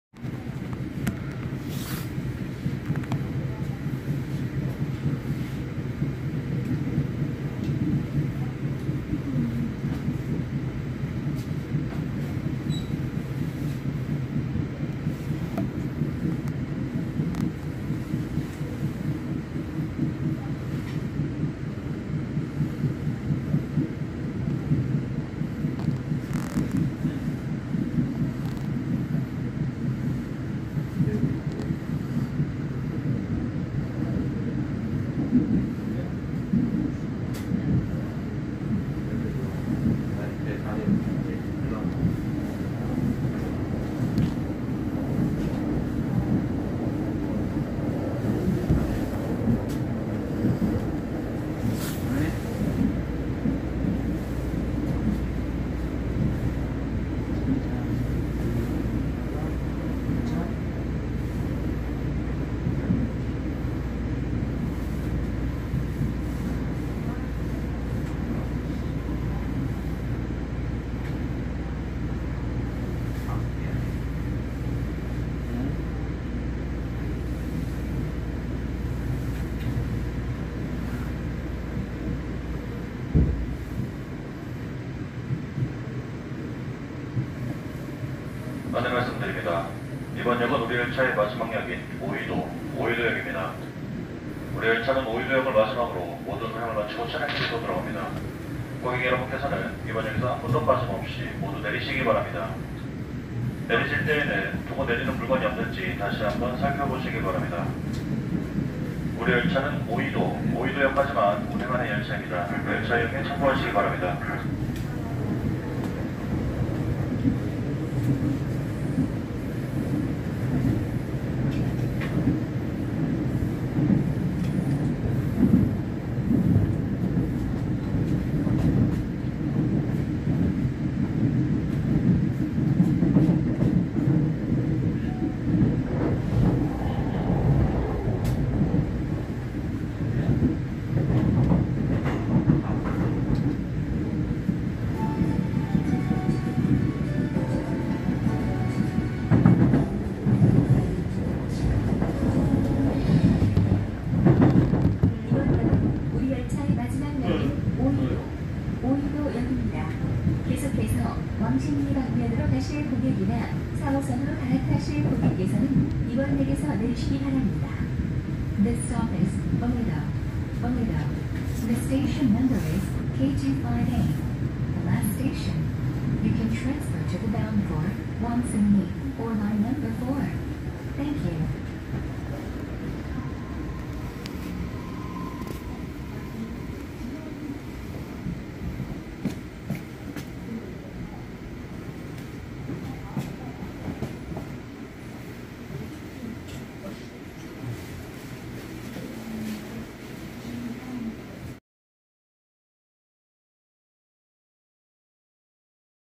수인분당선 K6750 오이도행 351X76편성 삼눈이 열차 시승기
중간에 395편성 큐브 납작이 열차 발견 되었다

마지막에 퀴즈가 있으니 정답은 댓글로 작성 해주세요